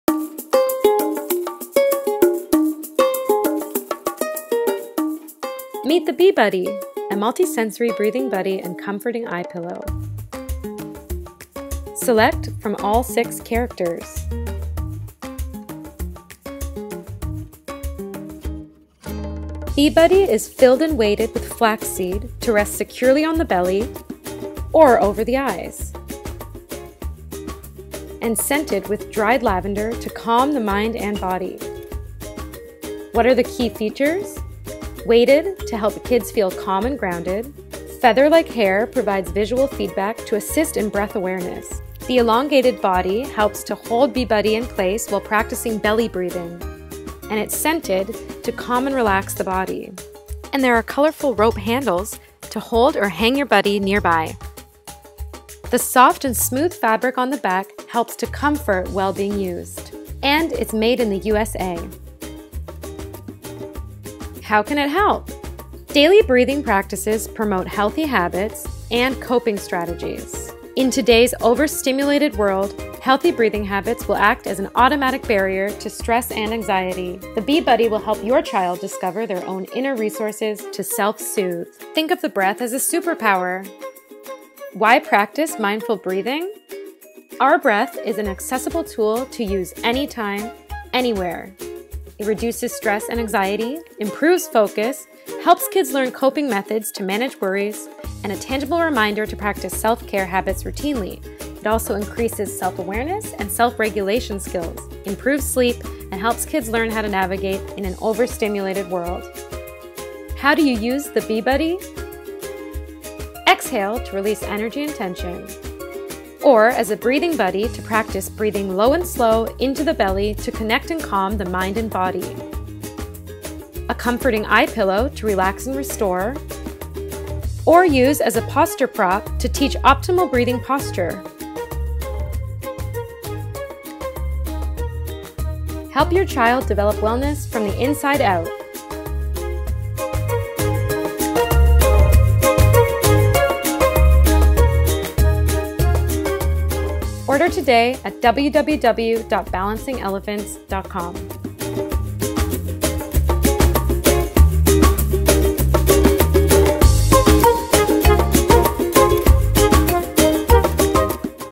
Meet the BE Buddy®- a multi-sensory breathing buddy and anxiety relief toy for kids!
The BE Buddy® is a multisensory breathing buddy & comforting eye pillow for children.  It is an animal-themed, breathing companion to empower kids to tap into their inner resources to manage stress, practice self-care habits, and cultivate a positive connection to themselves and others.

Use the BE Buddy® as a fun and engaging tool to practice healthy breathing habits to calm the nervous system and fuel the body the way it is designed.

BE Buddy® Key Features:
• Made in the USA • Made in Minnesota
• Canvas is made with 45% recycled content
• Feather-like hair providing visual feedback to assist in breath awareness
• Elongated body to hold in place while practicing belly breathing
• Brightly printed fabric offers visual stimulation, connection & personality
• Looped grip to aid in holding/hanging & additional tactile input
• Weighted with flax seeds to help kids feel calm & grounded
• Unique facial features to add character and lovable elements to the pillow
• Filled with dried lavender to calm and relax the body
• Soft & smooth fabric on the back to comfort while used as an eye pillow

BE Buddy® Practice BENEFITS:
• Multisensory to help kids pause and focus on the present and open awareness to their surroundings
• A comforting eye pillow to practice relaxation
• Conforms to the body without falling off
• Weighted to ground and comfort
• Promotes healthy breathing practices to safeguard children against the effects of stress and an overstimulated culture
• Teaches kids useful coping methods to manage worries
• A tangible reminder to practice self-care habits routinely
• Invites simple practices to attune children with their bodies
• No charging, no power, no apps required
• Offers a break from screen time